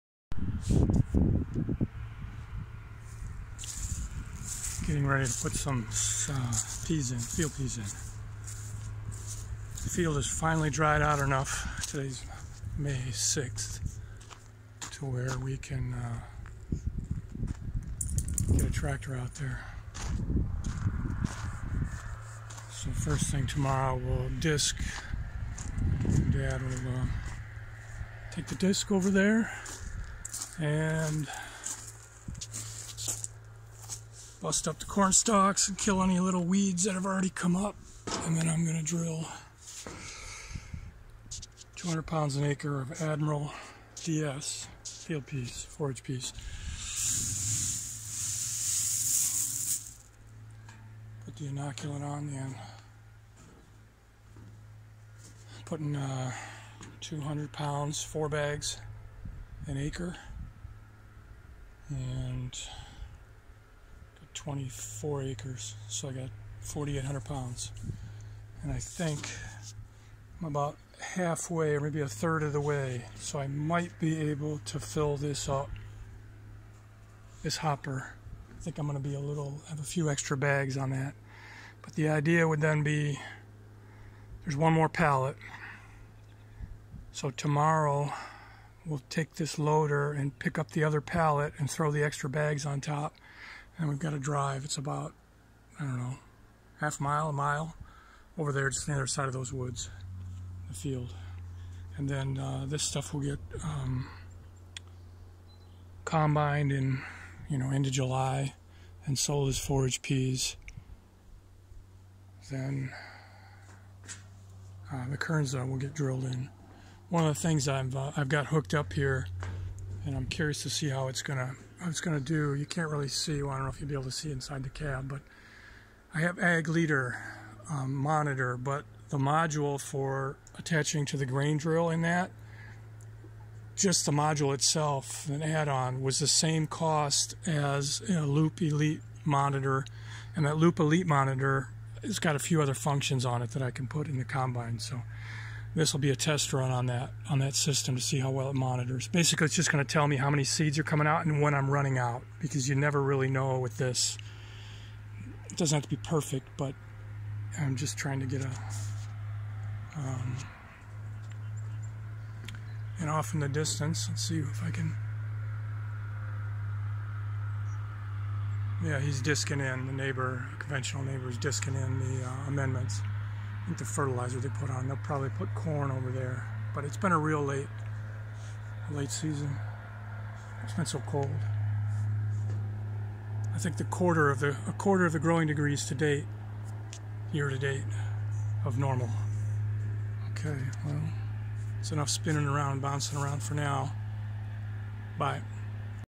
Loading Peas into Drill
I'm curious to see how the Loup monitor does on the seeds. I don't need it so much for the peas, I bought it mainly because of the Kernza. At ~ $700 per 50 lb bag, I want to make sure it gets in the ground right.